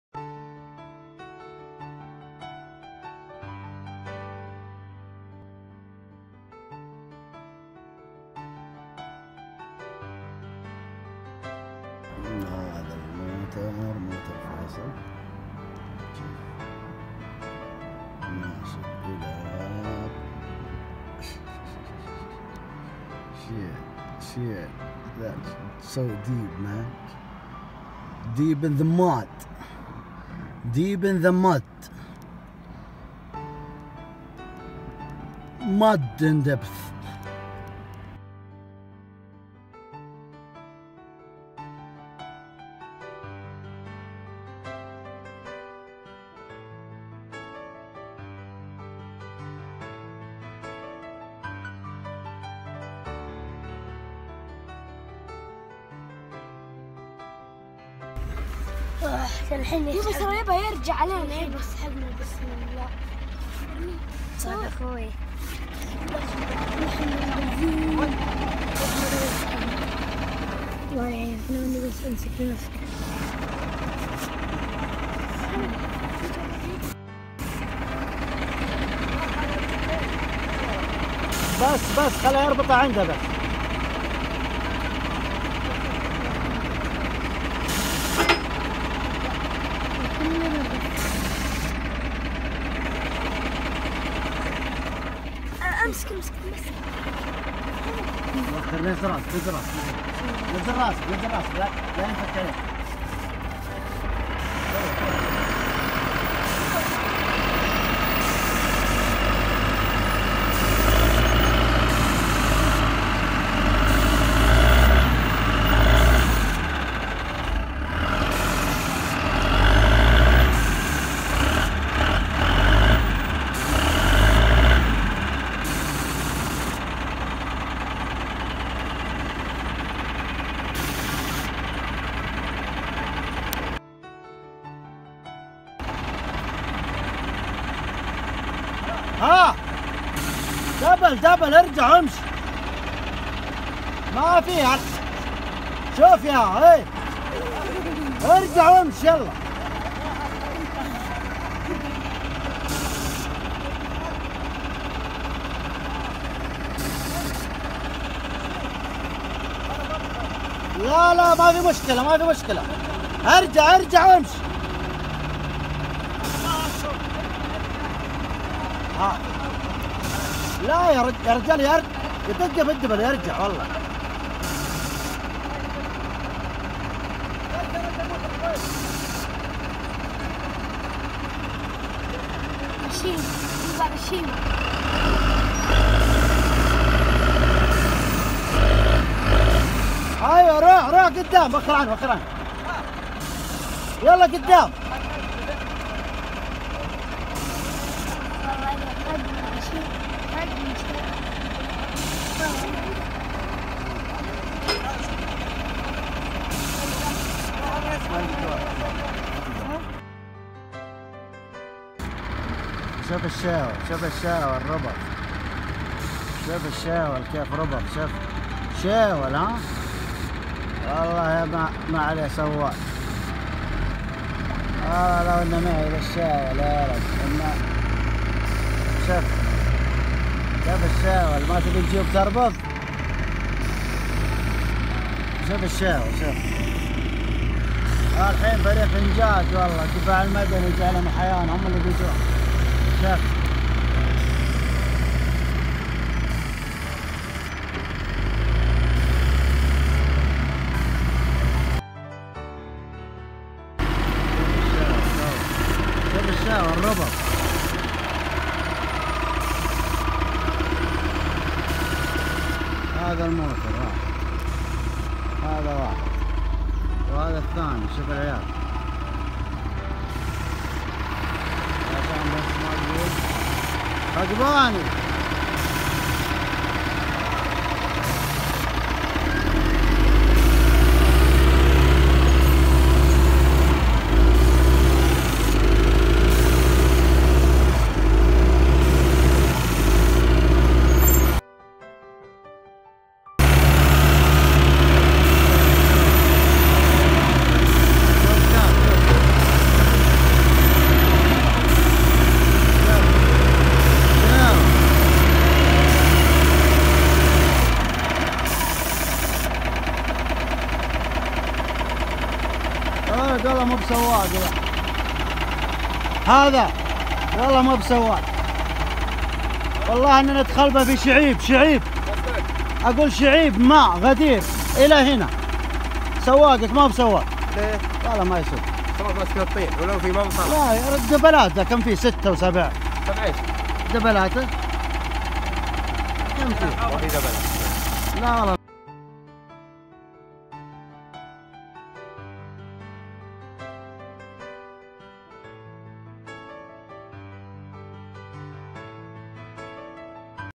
Extreem Off-roading in chewing deep mud in Saudi Arabia
Our 2 4wheels jeeps sucked by chewing mud in Saudi Arabia